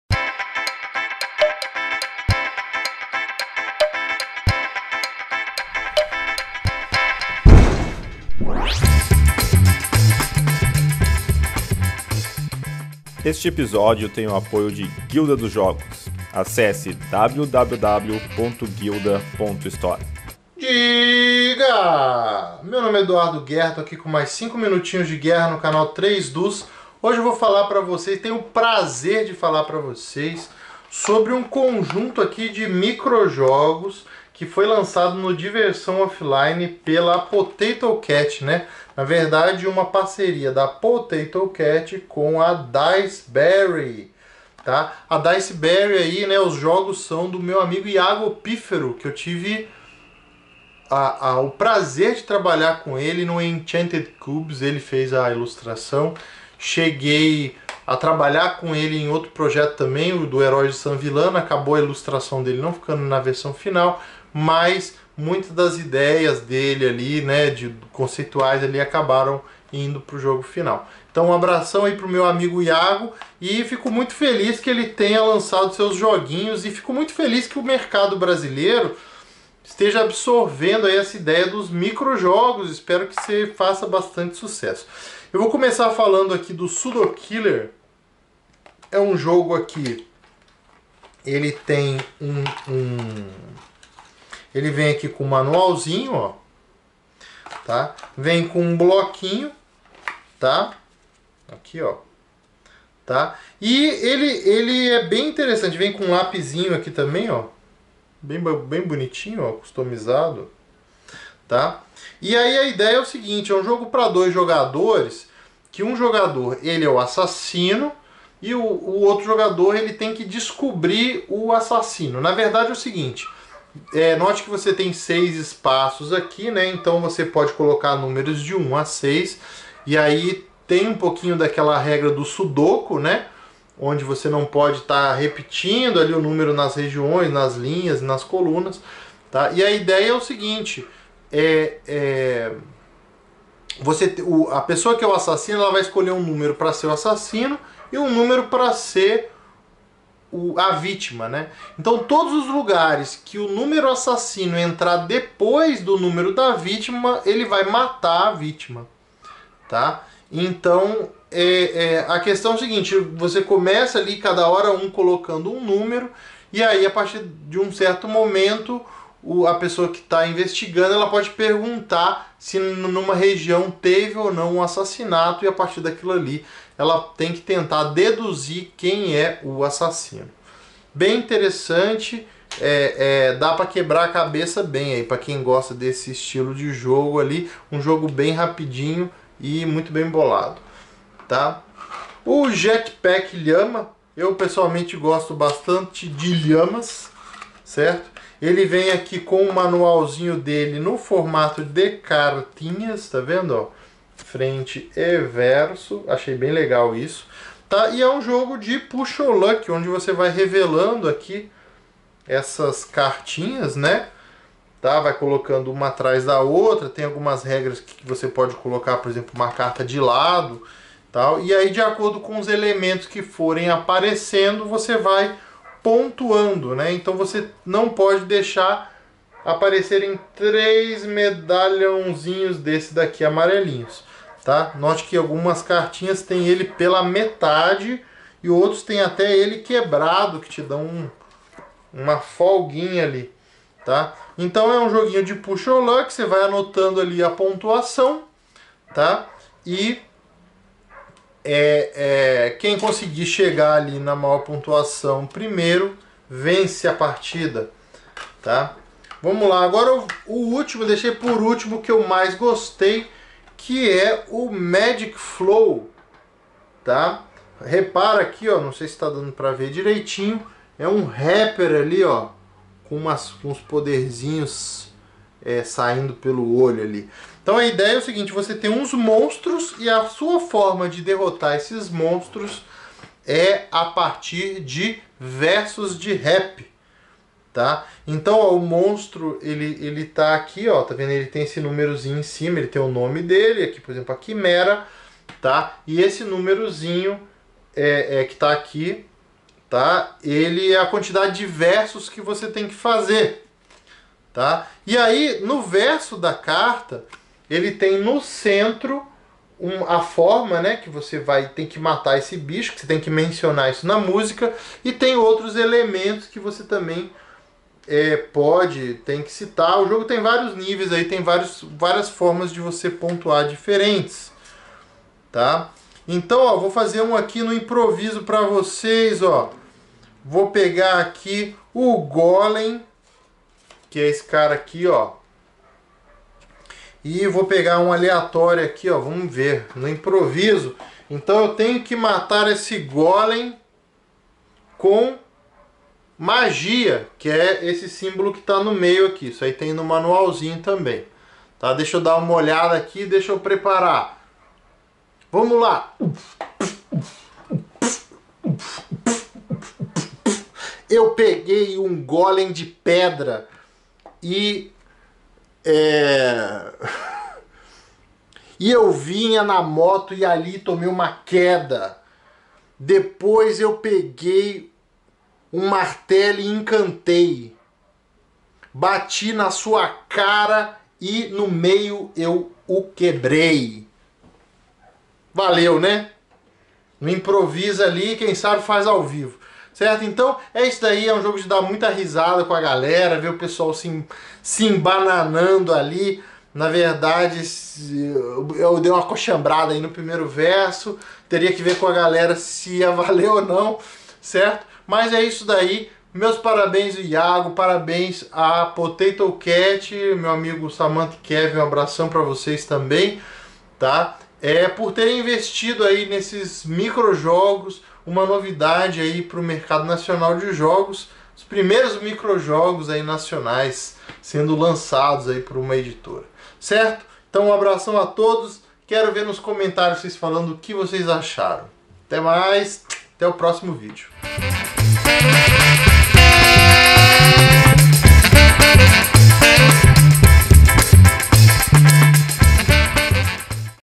5 Minutos de Guerra - Ep. 81 - Micro-Jogos da Potato Cat e Diceberry
Neste vídeo Edu Guerra fala de três micro-jogos que estão saindo no Brasil.